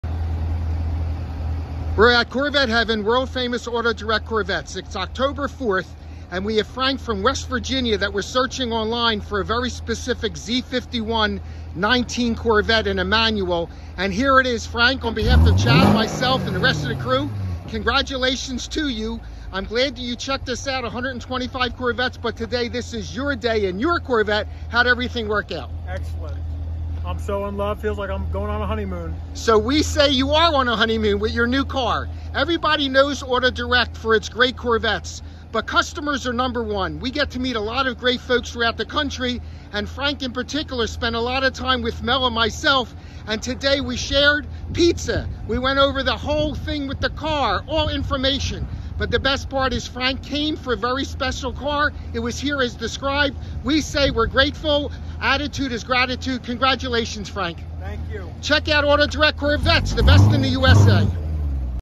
Corvette, Z51 Coupe
Going all the way to West Virginia. Congratulations to a great customer on the purchase of this beautiful Corvette Z51 coupe